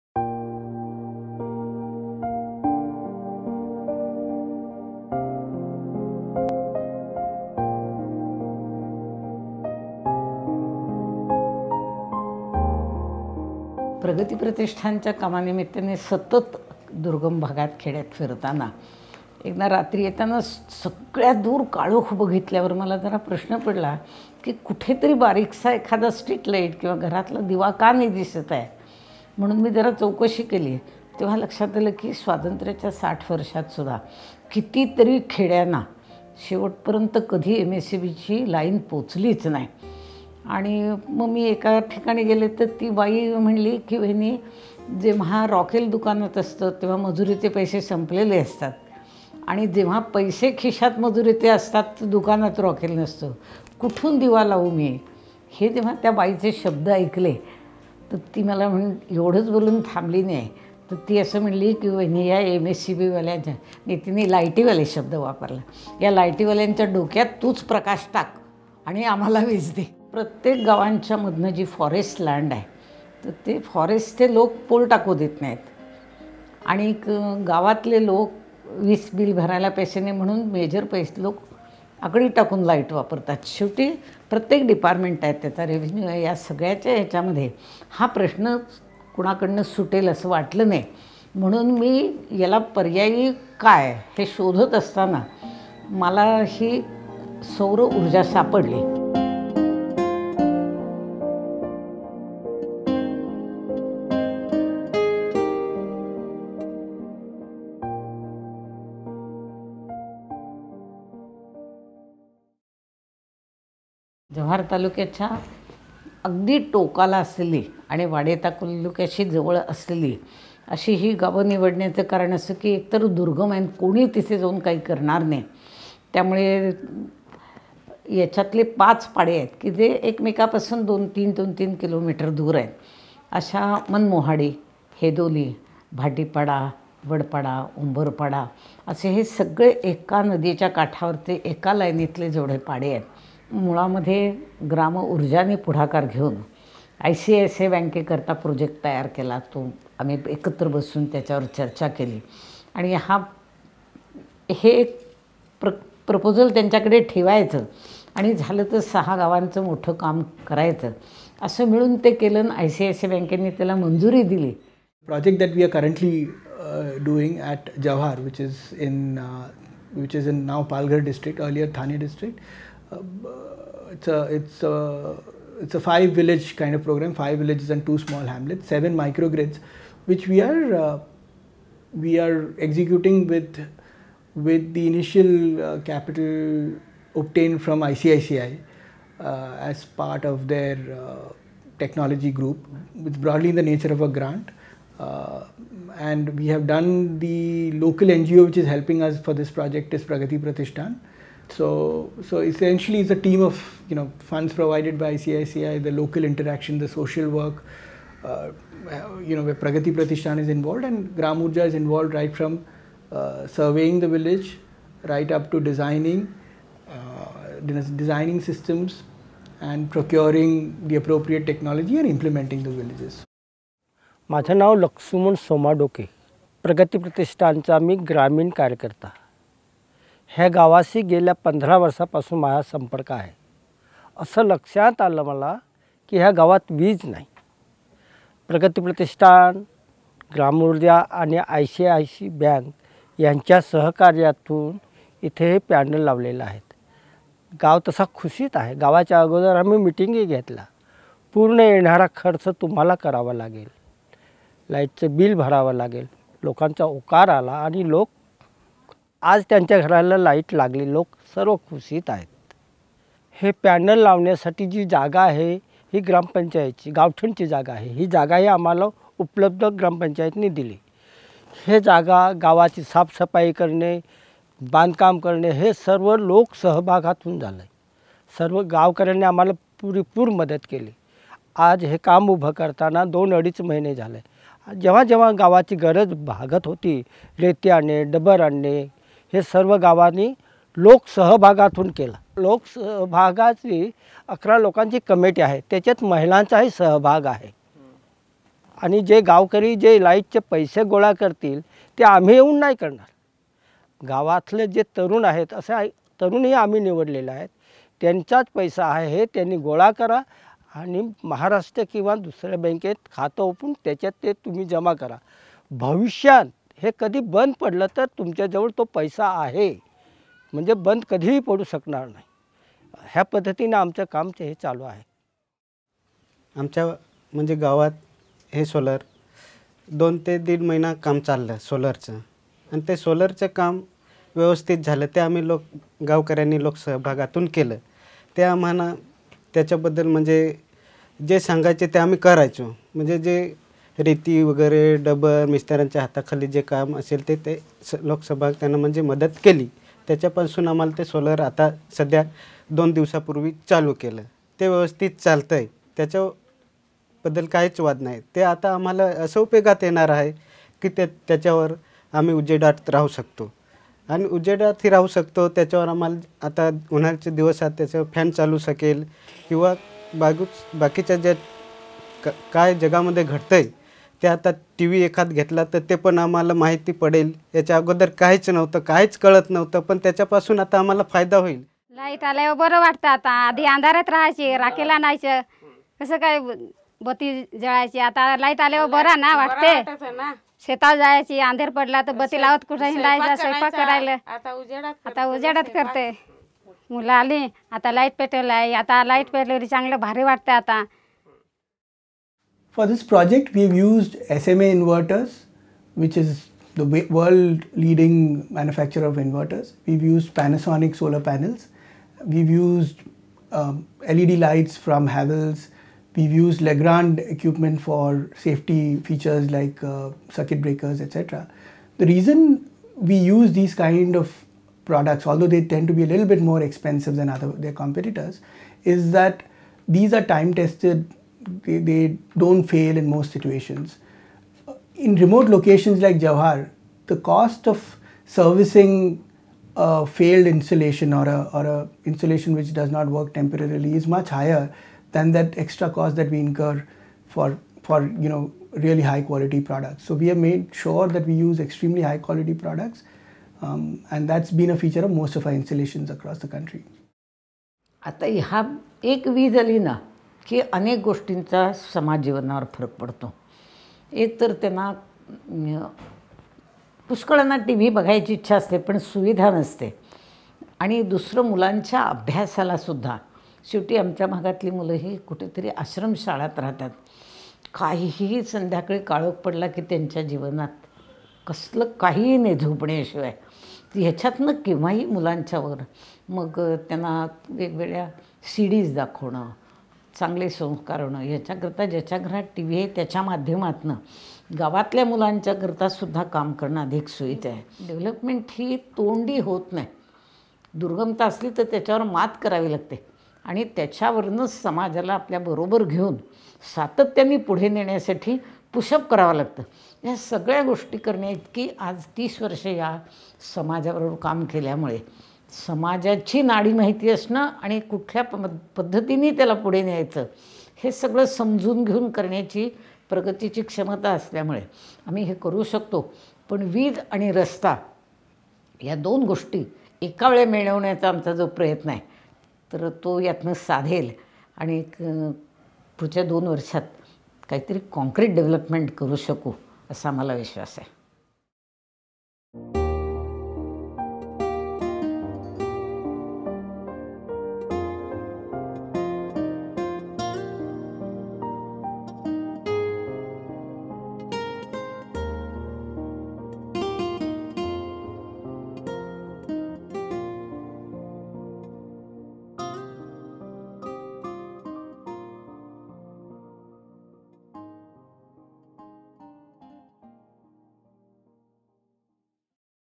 Powering Empowerment
A short film - 'Powering Empowerment' on solar minigrids in Jawahar, Maharashtra. This is a sustainable decentralised renewable project for unelectrified Indian villages. The project was made possible by the combined effort of gram oorja , pragati pratishthan and ICICI.